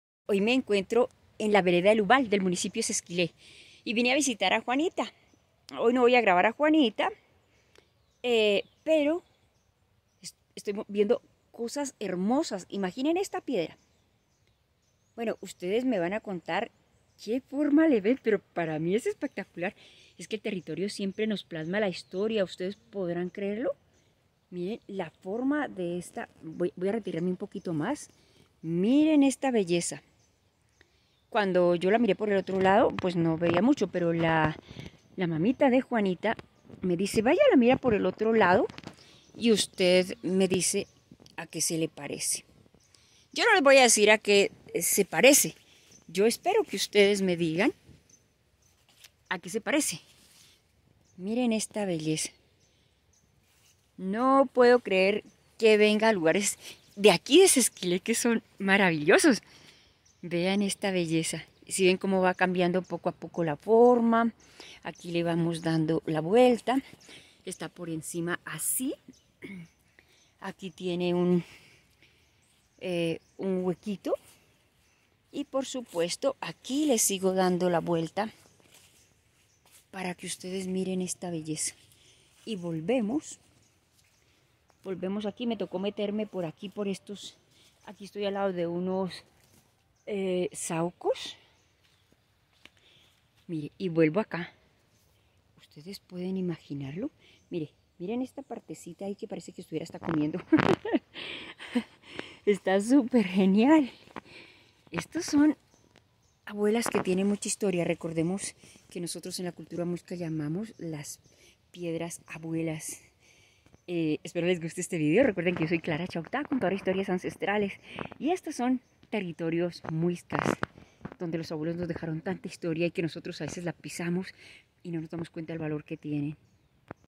UNA NIÑA, UN SAPO UN COCODRILO? MIRALO BIEN
Es una abuela (piedra) y tiene una figura tu ves su forma cuéntame, el territorio siempre nos plasma la historia